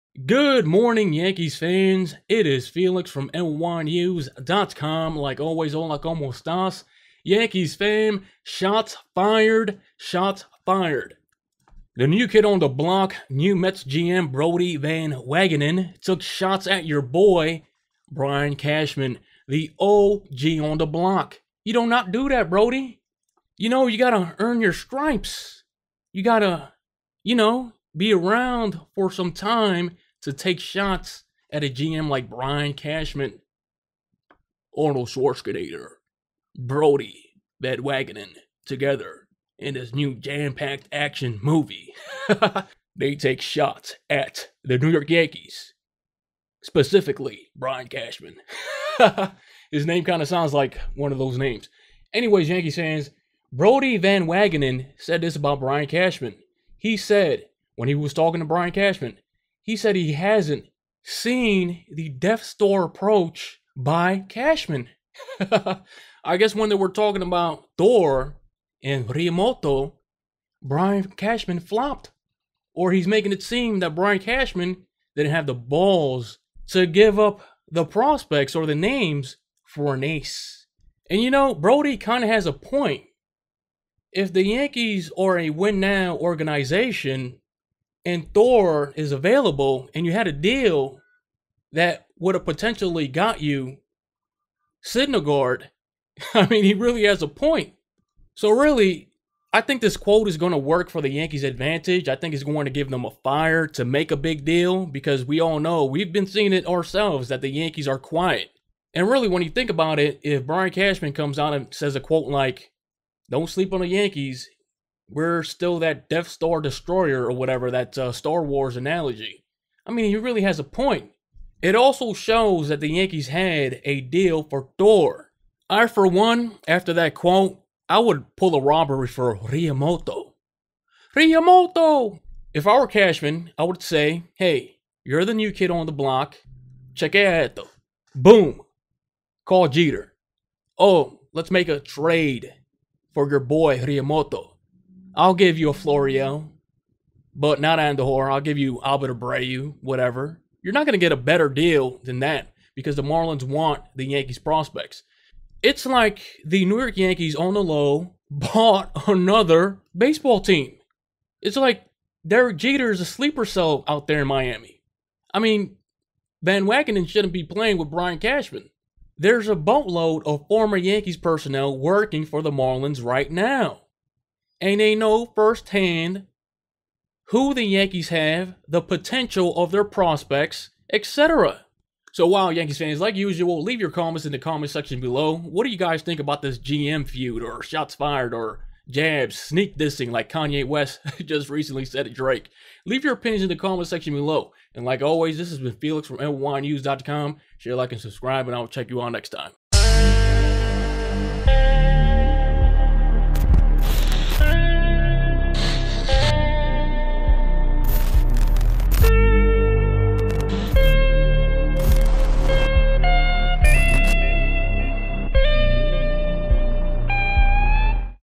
Shot's Fired, Mets GM Van Wagenen Disses Brian Cashman, How Will Cashman Respond? - Felix Pantaleon
mlb
yankees
mets
brodie van wagenen
brian cashman
j.t. realmuto
noah syndergaard
marlins
death star destroyer